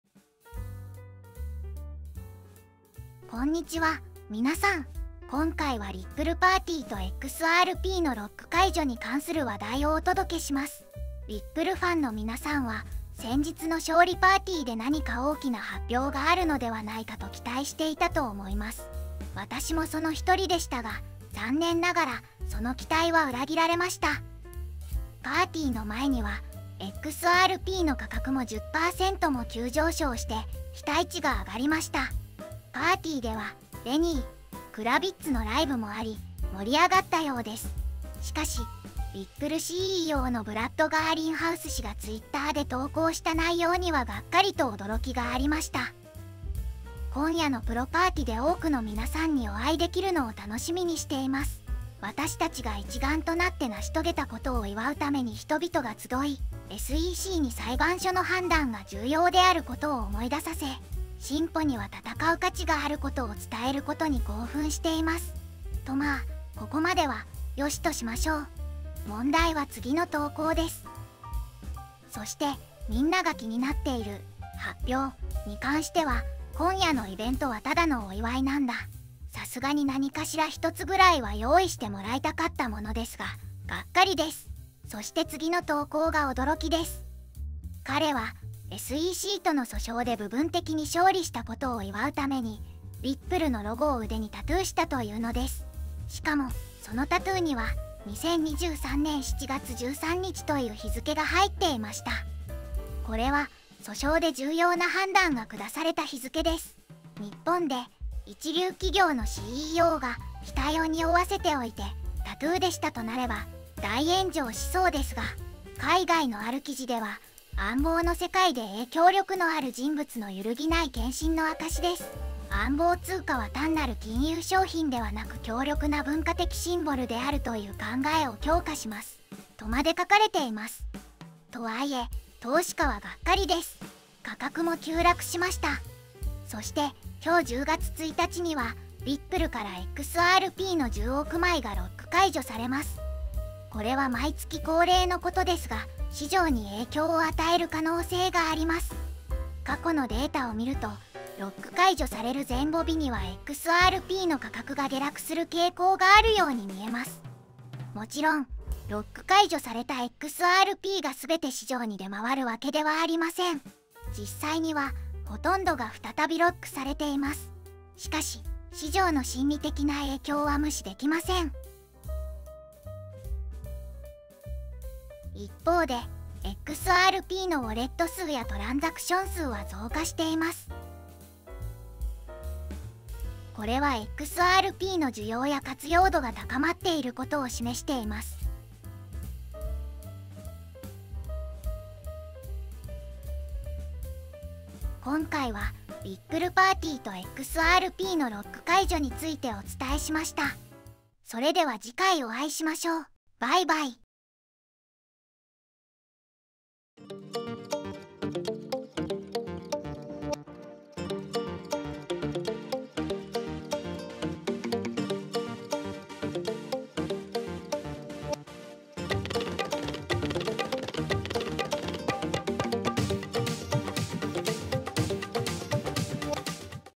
【リップル（XRP）】10億枚のロック解除がやって来る！【仮想通貨】リップルパーティーの顛末
今回はリップルパーティーとXRPのロック解除に
関する話題をお届けします。
リップルCEOのブラッド・ガーリンハウス氏が
なんとタトゥーを刻印。
10億枚の開放で過去はどうなった？確認してみた。

2023年9月30日までの情報で作成

重要↓
※情報の提供であり投資アドバイスではありません
※投資は自己責任でお願いいたします。

・コインマーケットキャップ　ビットコイン
ー－－－－－－－－－－－－－－－－－－－－－－－－－－－－－－－